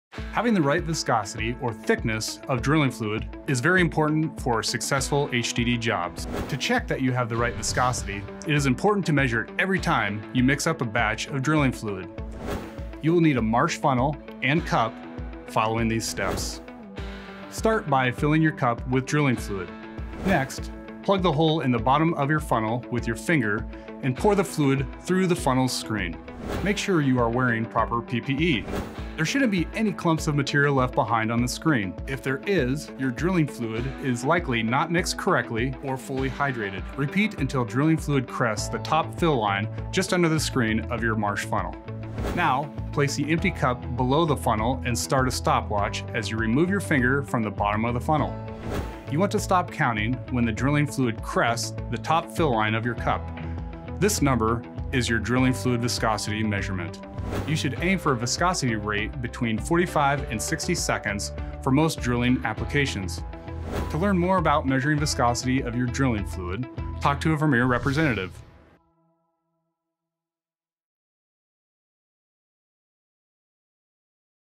How to measure drilling fluid viscosity
Learn how to mix your drilling fluid as Dan Vroom, a crew skills training manager at Vermeer, talks about the importance of measuring the thickness of drilling fluid, and the best way to do it.